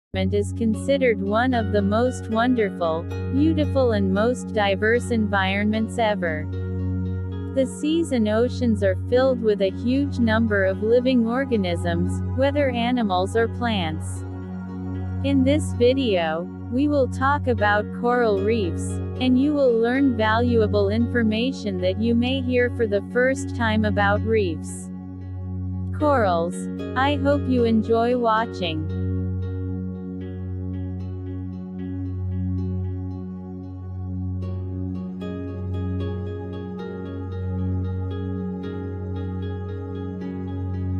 beautiful coral reefs
Under the Red Sea 4k_Incredible underwater world_Coral reefs dazzle the eyes.
A vast yet majestic and vibrant underwater journey to the world's most beautiful lakes.

Filmed in high definition, this video offers magical glimpses of an underwater world full of life, color and beauty.
 Relaxing beautiful reef fish.
Helps fall asleep quickly.
 Calm music for relaxation, sleep, meditation, yoga and study.
 # Animals kingdom
# Animals around The world
# Animals world
# sea world
# underwater life
# coral reefs.
In this video, you will learn about the importance of coral reefs, their types and where they are located.
Characteristics of coral reefs and their benefits and harms.
Why is it called by this name.
How to preserve and protect them from lending.